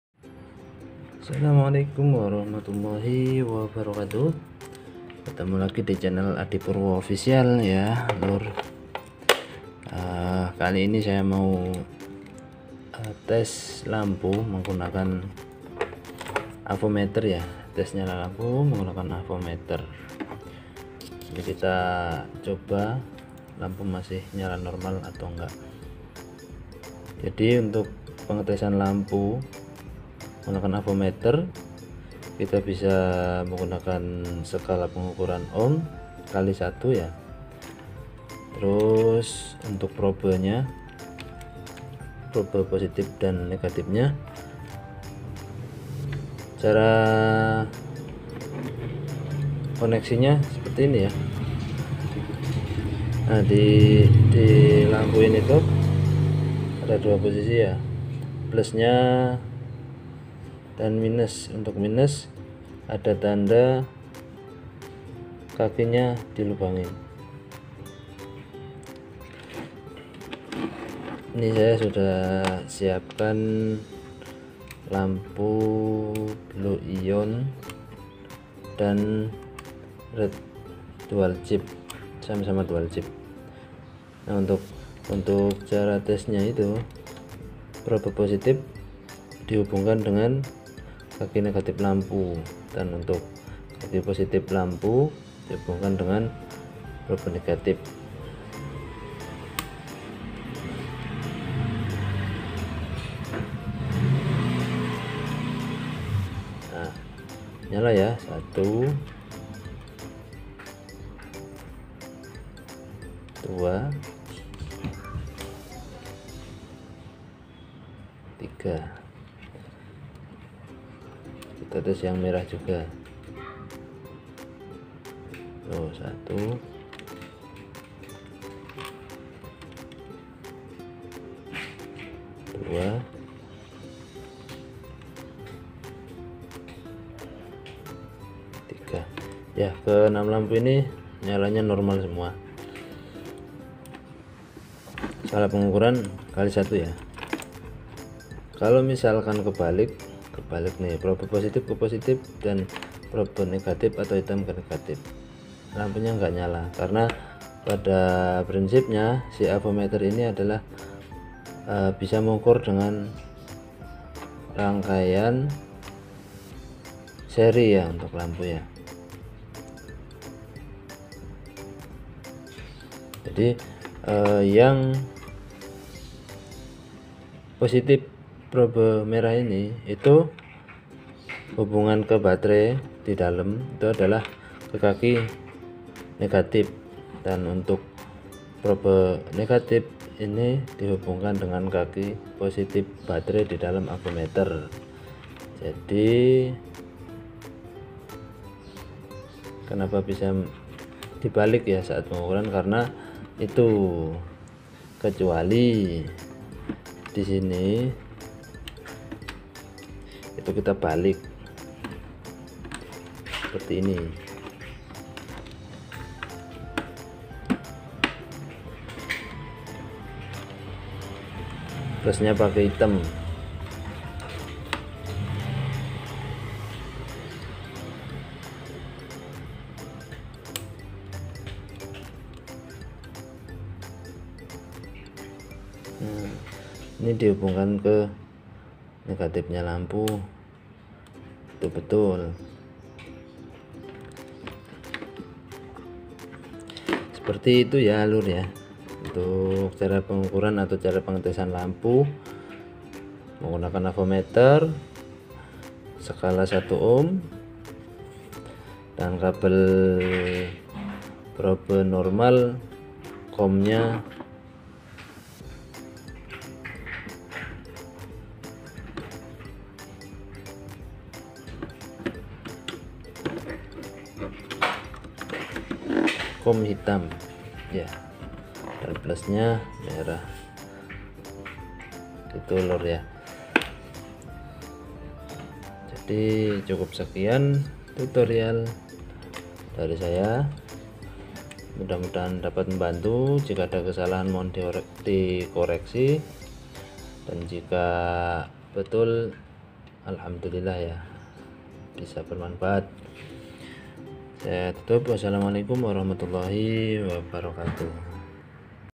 TUTORIAL AVOMETER UNTUK TEST LAMPU LED HPL 3VOLT
AVOMETER
OHMMETER
SKALA AVOMETER
TEST LAMPU LED MENGGUNAKAN AVOMETER SKALA OHM X1 UNTUK MENGETAHUI LAMPU MASIH NYALA ATAU TIDAK
PERHATIKAN TERLEBIH DAHULU PROBE / KABEL AVOMETER, PIN/KAKI LAMPU LED